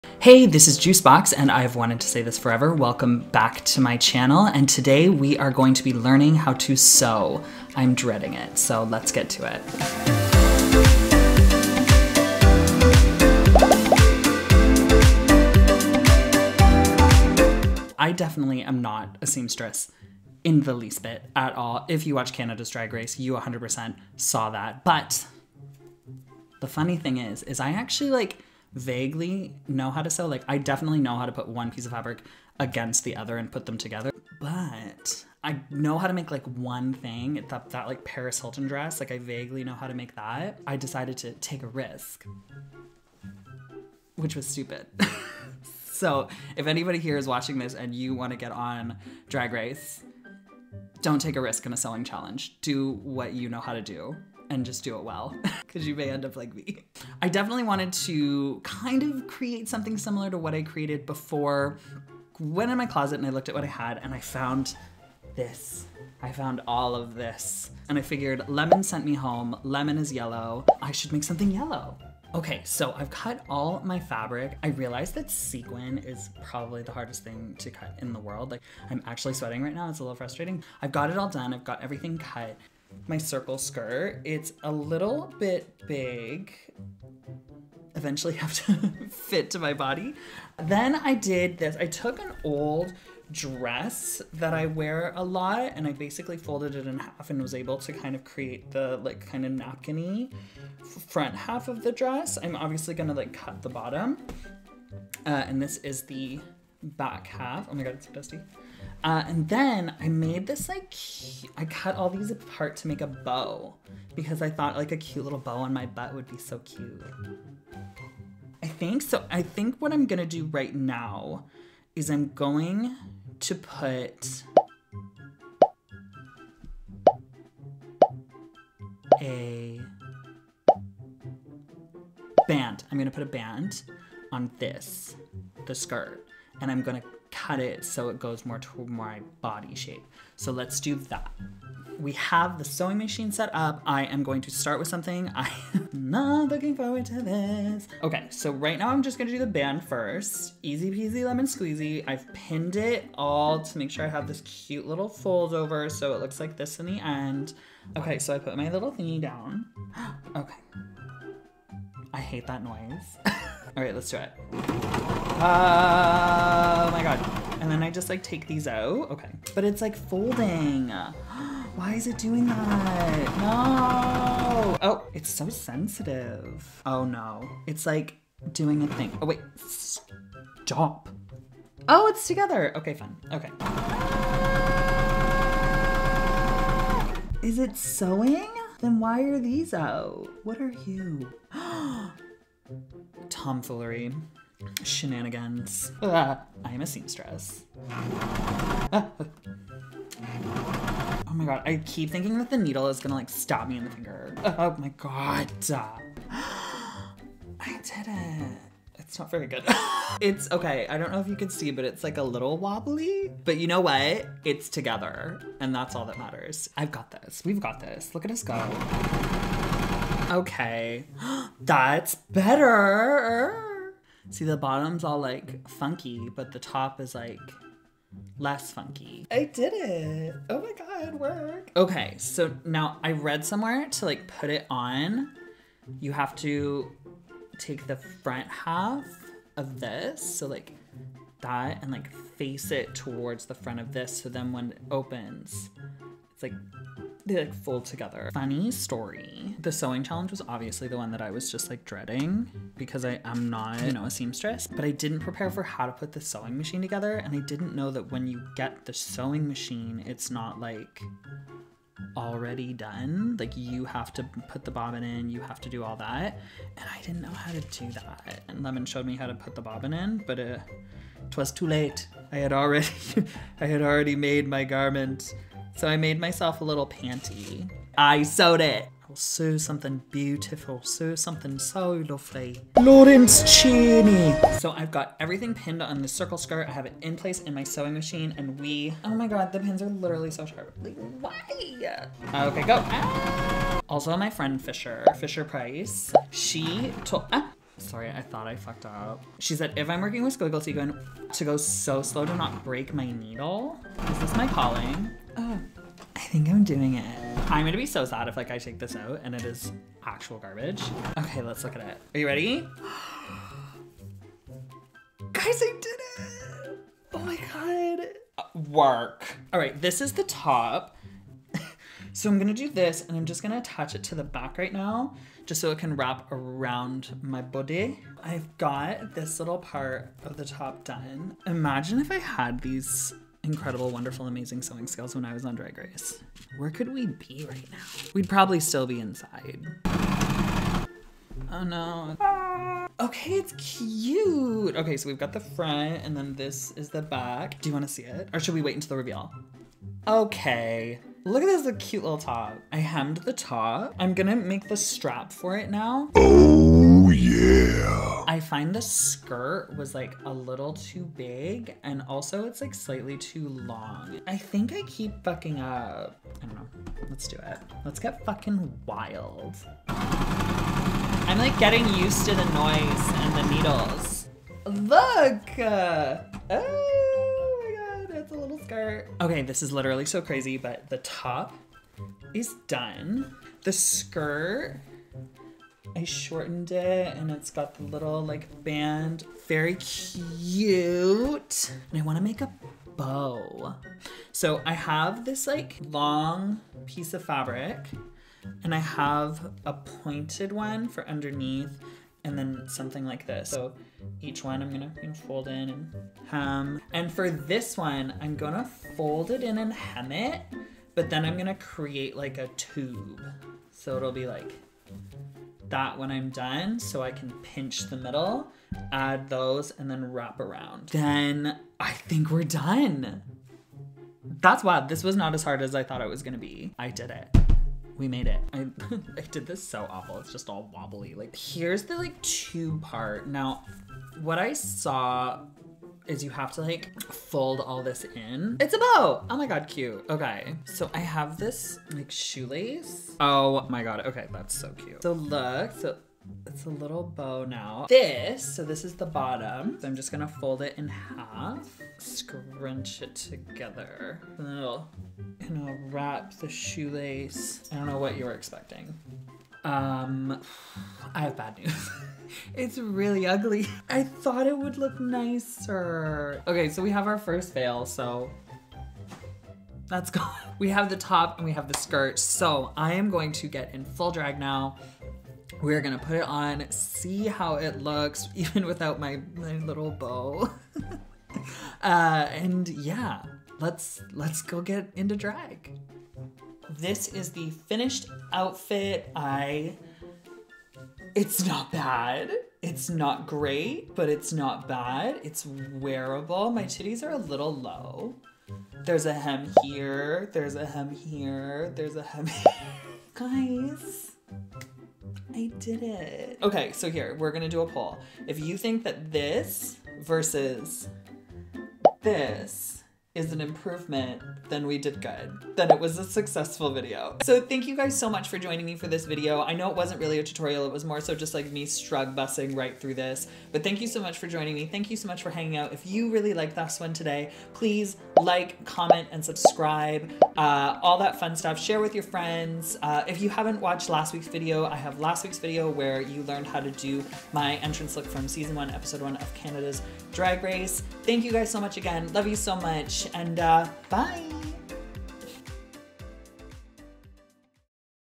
Learning How to Sew!
Watch me struggle through learning how to sew :/!

Tip me!

Opening Graphic: Alex Josselyn